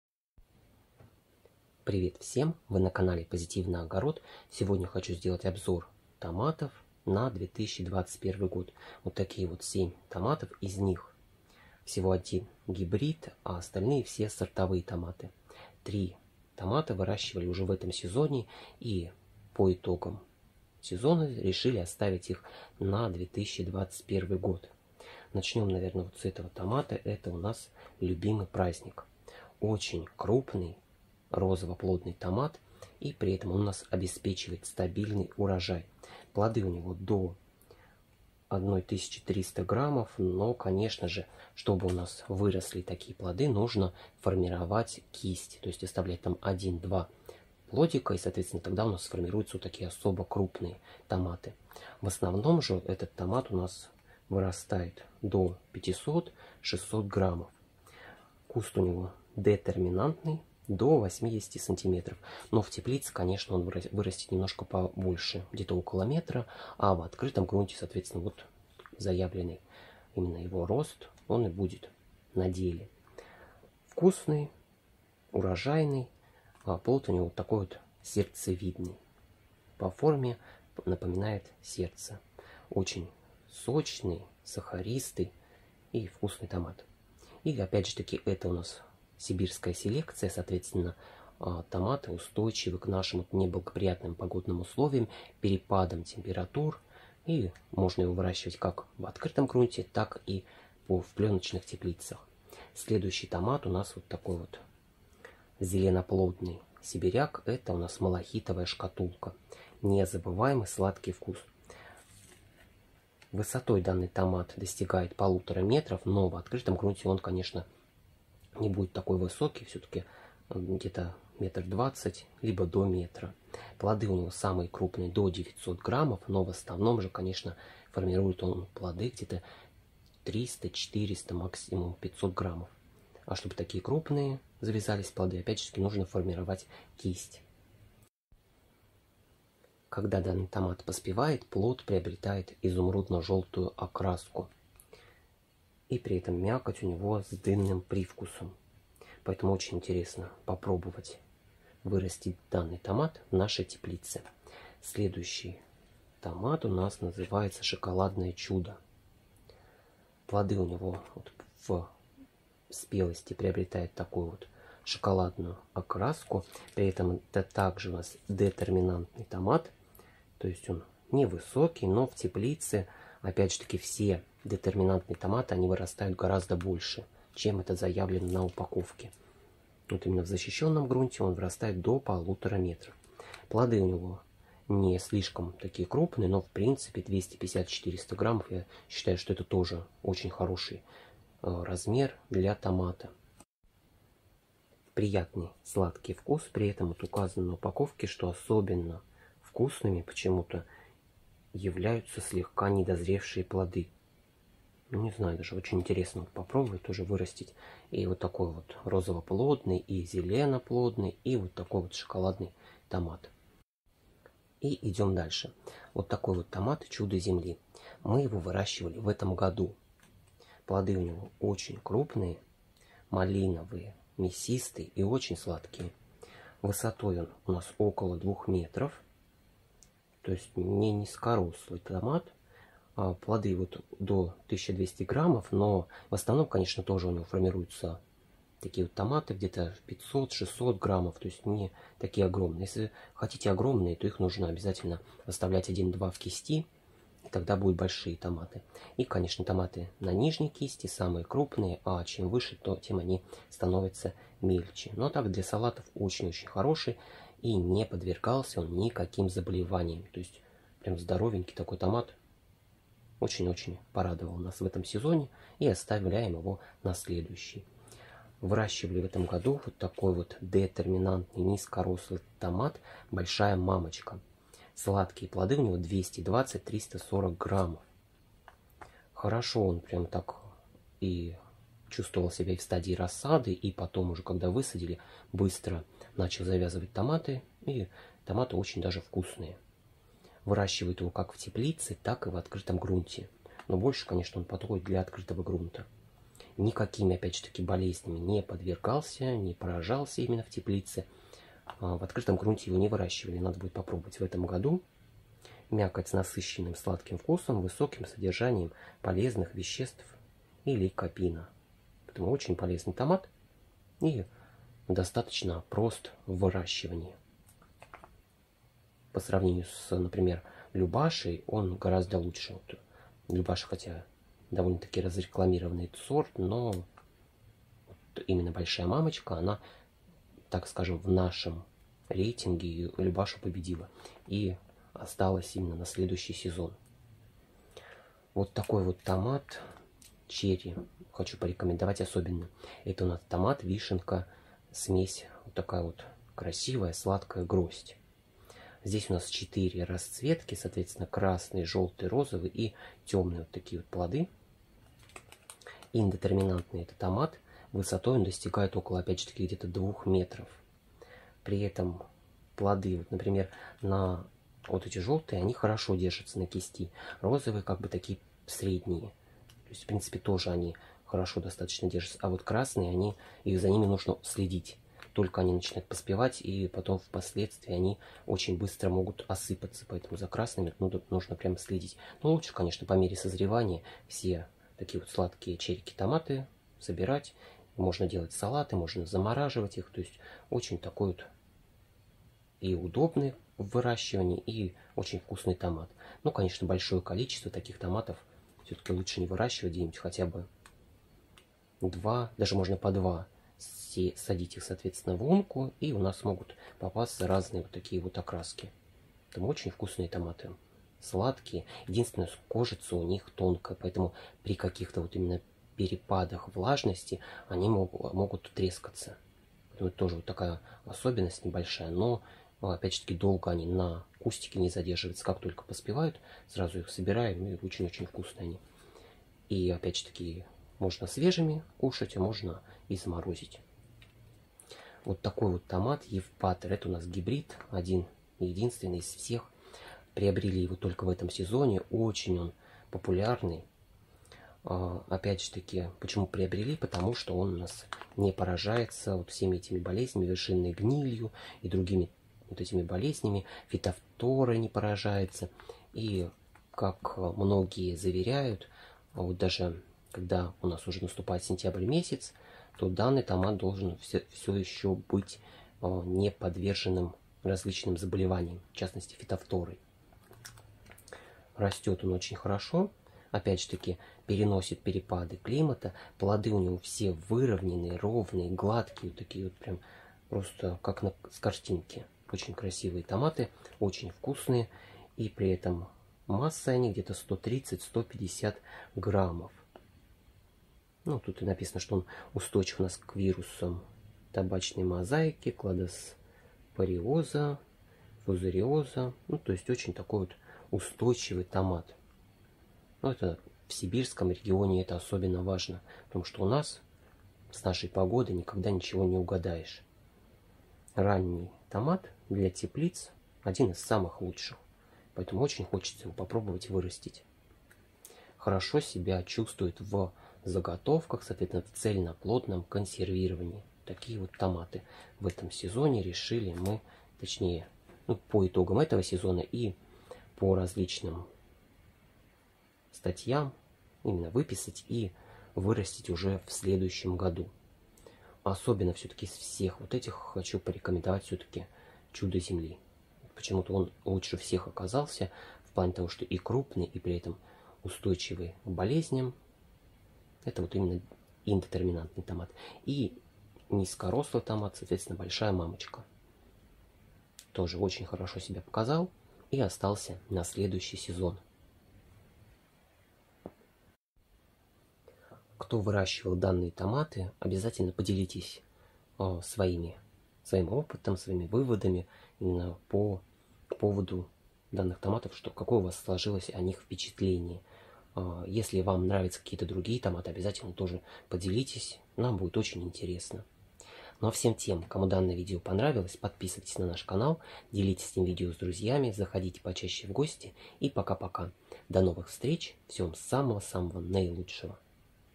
Обзор семян томатов на 2021 год. Крупноплодные помидоры.
Томаты которые планируем выращивать в следующем сезоне: четыре новинки и три сорта помидоров из этого сезона, которые показали отличные результаты. Урожайные, крупноплодные томаты, которые можно выращивать, как в теплице, так и в открытом грунте. И один черри-томат, очень сладкий, вкусный, различной окраски, хорош и в свежем виде и в заморозке.
Томаты:
- Любимый праздник,
- Малахитовая шкатулка,
- Шоколадное чудо,
- Чудо земли,
- Большая мамочка,
- Евпатор F1,
- Вишневая гроздь.
Надеемся, что данные томаты порадуют нас в следующем году. Посмотрим, какой они нарастят урожай. Также нам было бы интересно узнать, какие вы выращиваете сорта и гибриды томатов, какие получили результаты. Будим очень рады, если поделитесь своим опытом!